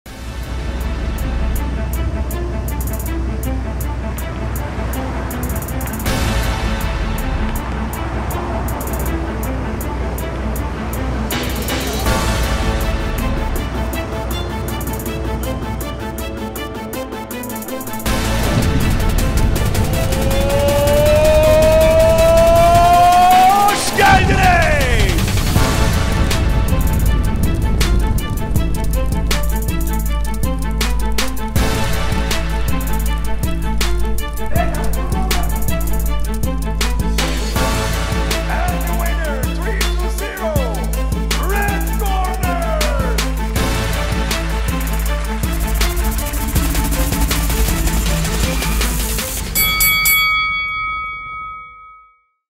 Army Of Fighters 4 Organizasyon Jeneriği
ARMY OF FIGHTERS EFSANE KADROSUYLA GERİ DÖNÜYOR!
19 Aralık Pazartesi - Army Of Fighters 4 Fragmanı
Konum: Şişli Grand Cevahir Hotel
Saat: 21.00
Kanal: TV 8,5